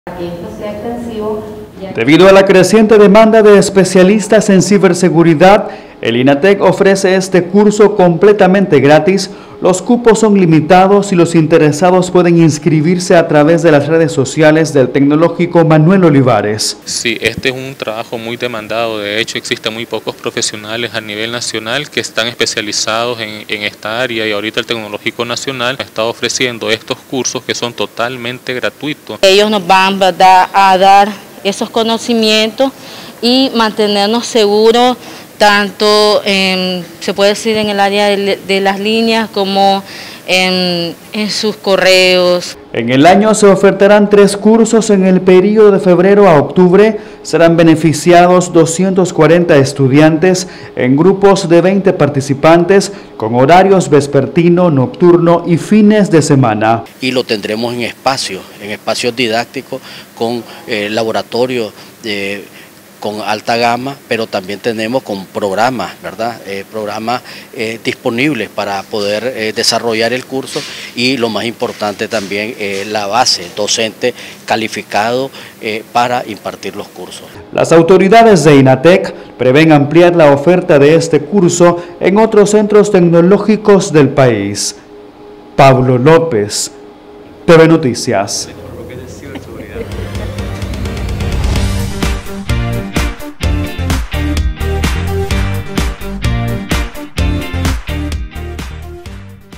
Inicia curso de ciberseguridad en centro tecnológico Manuel Olivares
Autoridades del Tecnológico Nacional (Inatec) anunciaron que ya se está ofertando el curso especializado en “Técnicas de Protección de Ciberseguridad”, las clases serán impartidas en el centro tecnológico Manuel Olivares Rodríguez de Managua.